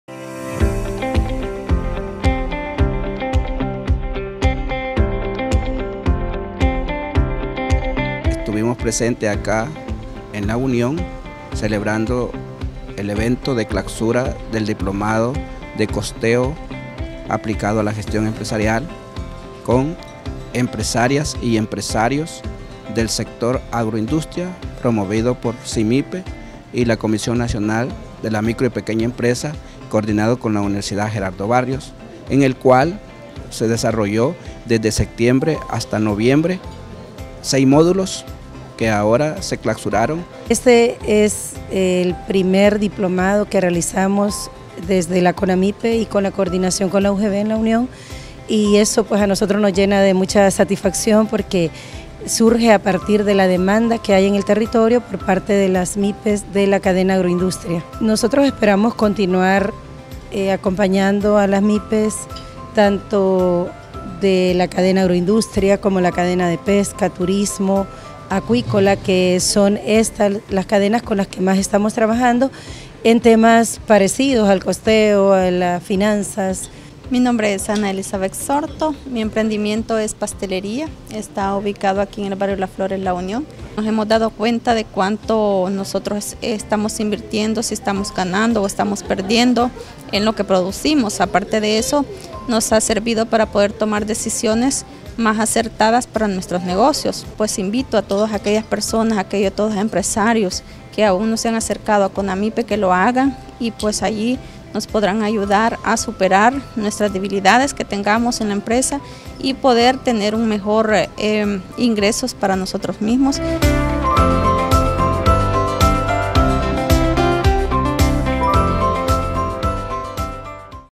Costeo aplicado a la gestión empresarial
No te pierdas nuestro video resumen del diplomado: “Costeo aplicado a la gestión empresarial” realizado por CONAMYPE y el Centro de Desarrollo de Negocios UGB